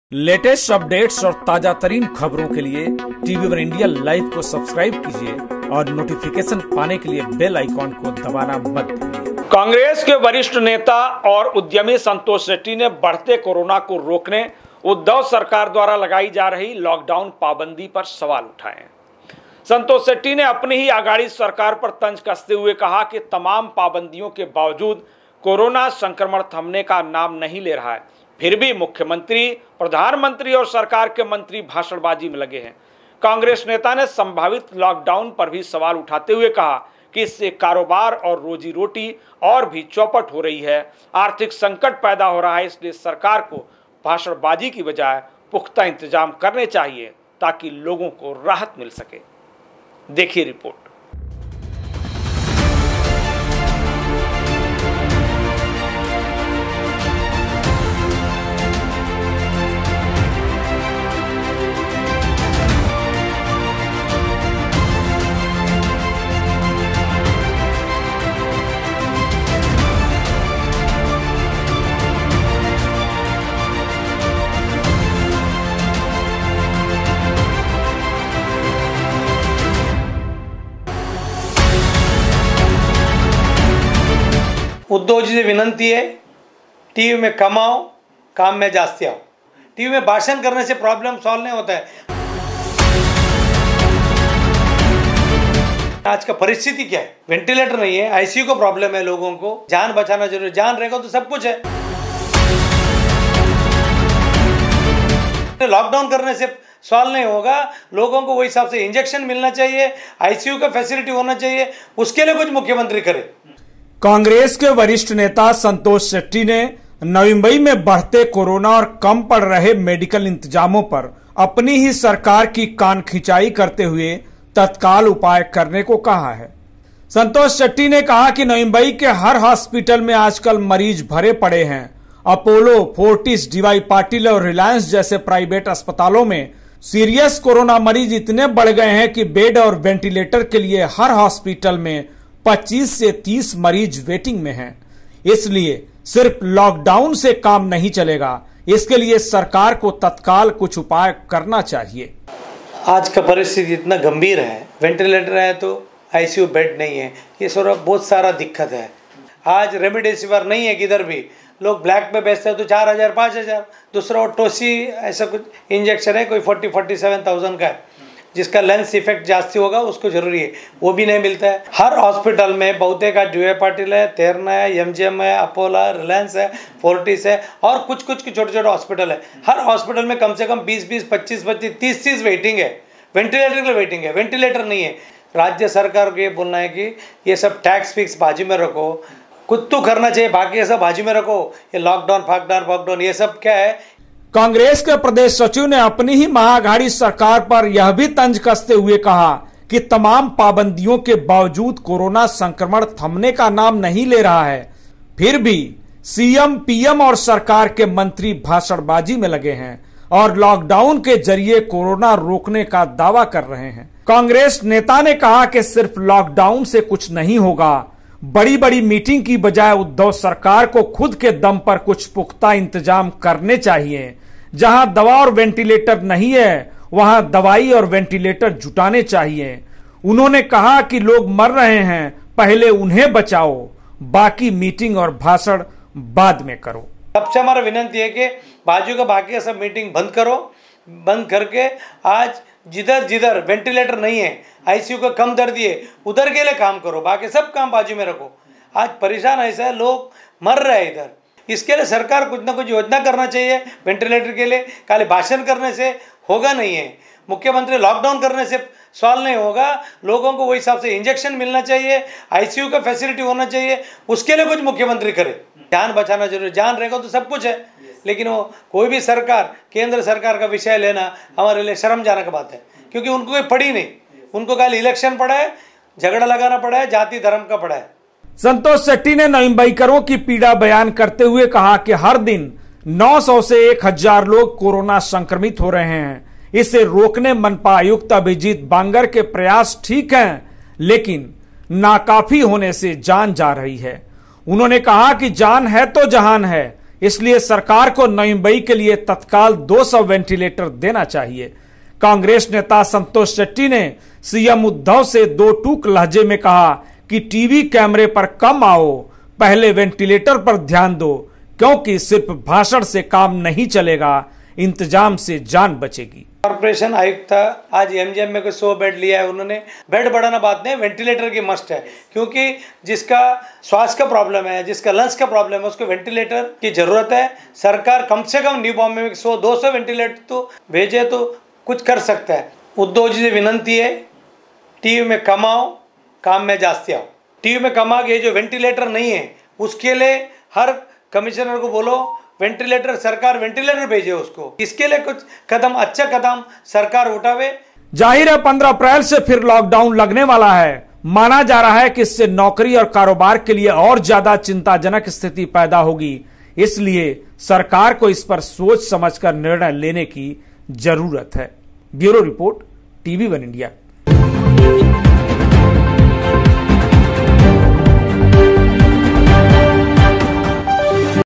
Congress नेता संतोष शेट्टी ने Lockdown पर उठाए सवाल
कांग्रेस के वरिष्ठ नेता और उद्यमी संतोष शेट्टी ने बढ़ते कोरोना को रोकने उद्धव सरकार द्वारा लगाई जा रही लॉकडाउन पाबंदी पर सवाल उठाए हैं. संतोष शेट्टी ने अपनी ही आघाड़ी सरकार पर तंज कसते हुए कहा कि तमाम पाबंदियों के बाजजूद कोरोना संक्रमण थमने का नाम नहीं ले रहा है.